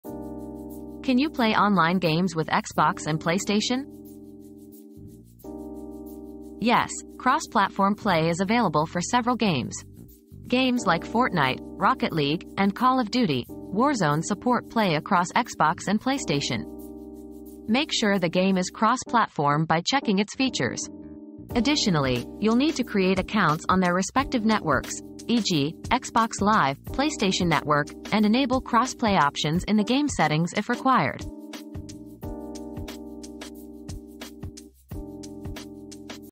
Can you play online games with Xbox and PlayStation?
Cross-Platform Gaming: Xbox vs PlayStation • Cross-Platform Gaming • Discover how you can play online games with Xbox and PlayStation through cross-platform play. Games like Fortnite, Rocket League, and Call of Duty: Warzone support this feature. Get ready to compete with players on both consoles!